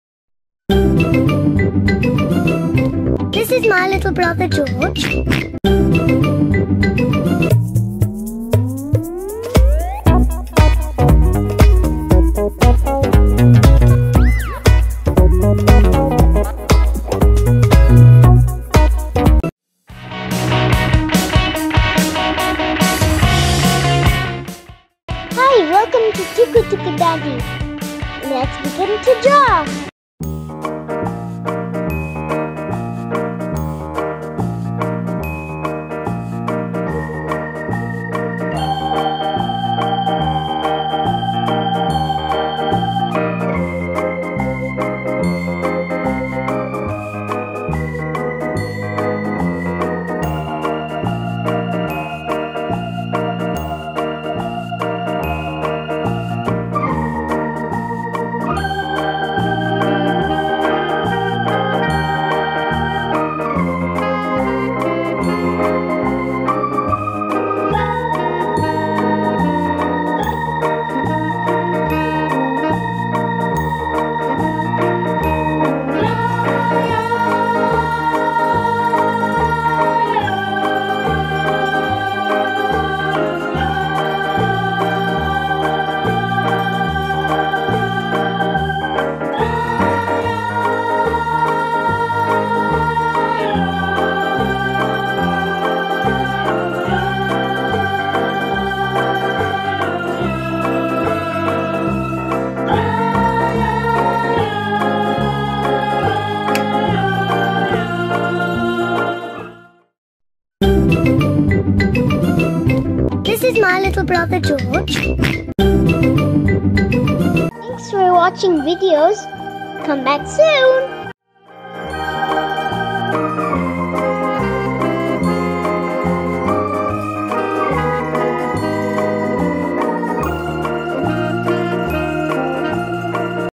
How to draw Geroge Pepa pig |Easy Geroge drawing | tuku-tuku daddy Drawing 14| Pepa pig drawing 2
How to draw george - Pepa pig , Simple and easy george drawing,  learn to draw george Pepa pig , lets color it and glitter it and then incorporate the drawing into some real world images.